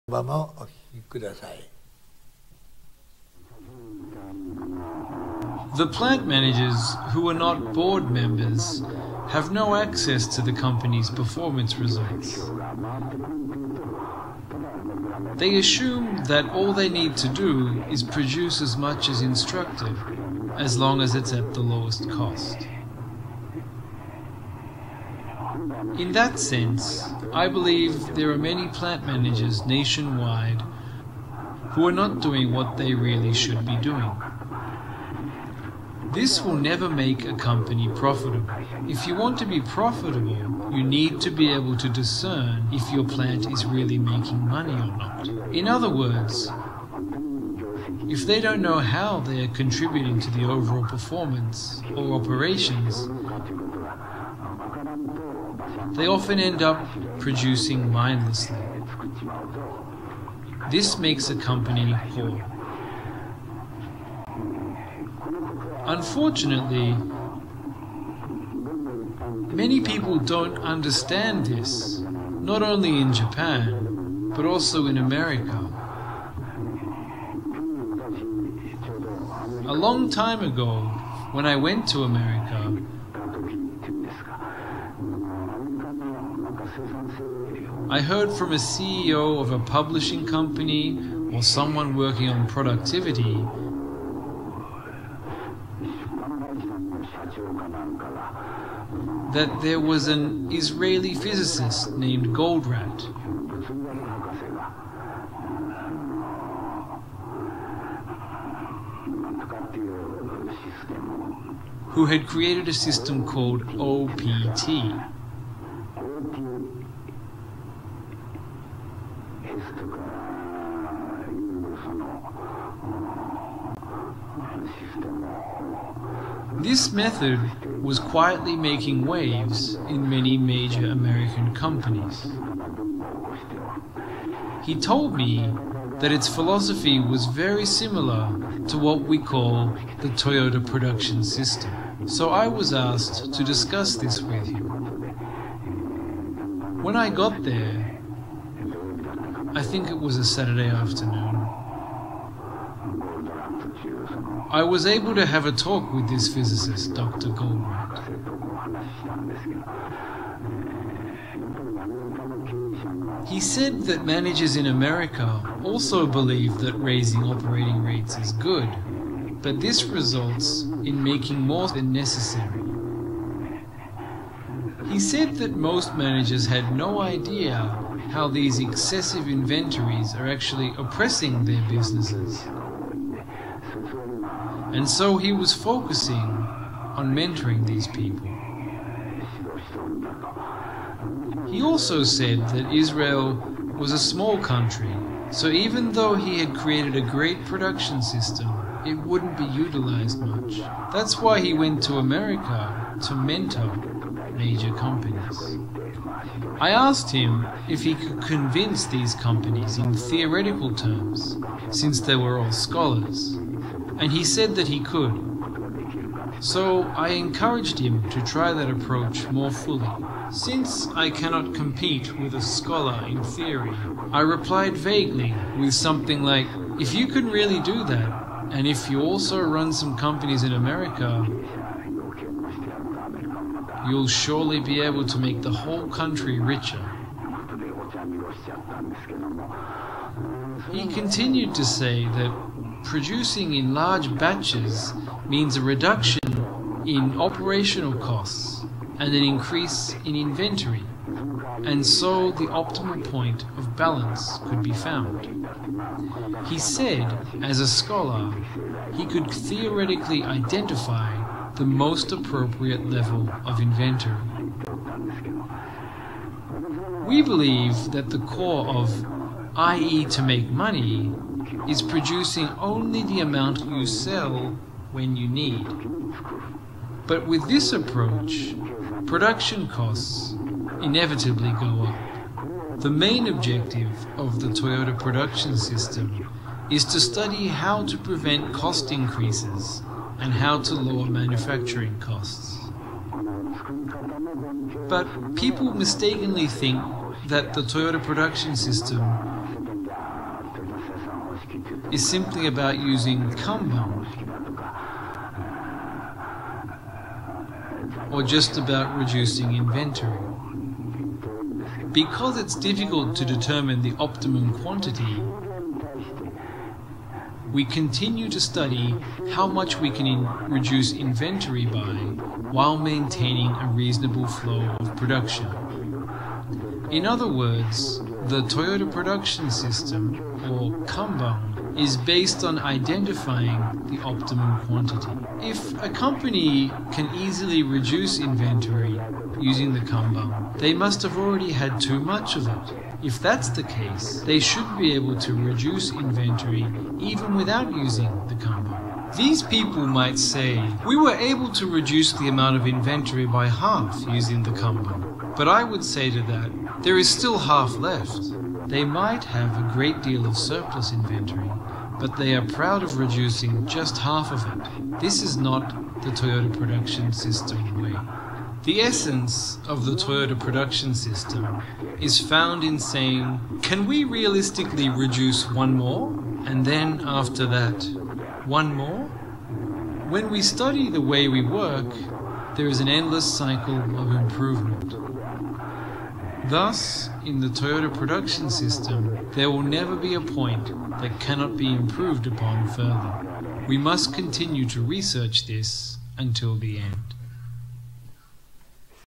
Ohno about the differences with the Theory of Constraints of Goldratt as both discuss in the past
Taiichi Ohno talks about his thoughts about the conversation that he and Eli Goldratt hold in the past about the similarities and differences of both systems. Original source can be found in this